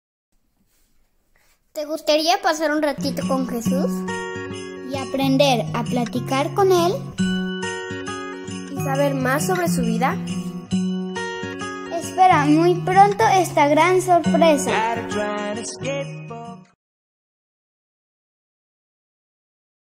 GodCast Presenta... GodCast Kids!
GodCast Kids! El Podcast para niños y niñas que quieren rezar y aprender Un Ratito con Jesús.
¡Espéralo todos los Martes a partir de éste 21 de Marzo!
Estará disponible en Youtube, Spotify y Apple Podcast.